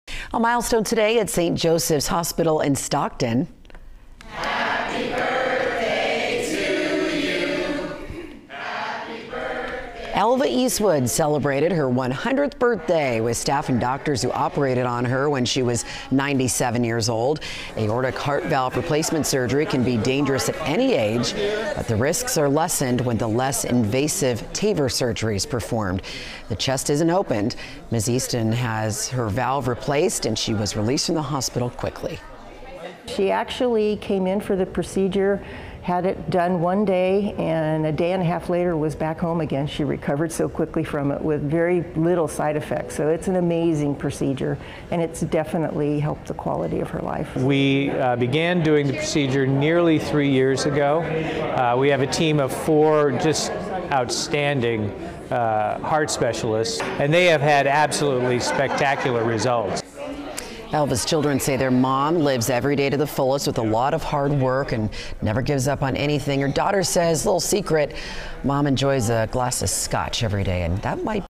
Heart surgery patient celebrates 100th birthday in Stockton
A heart surgery patient celebrated her 100th birthday Thursday at St. Joseph’s Hospital in Stockton Elva Eastwood was 97 when she needed to have her aortic heart valve replaced. The surgery used to be more evasive. But, a new method that doesn’t require the patient’s chest being opened is less risky. Eastwood was quickly released from the hospital after her surgery. Get the full story in the video above.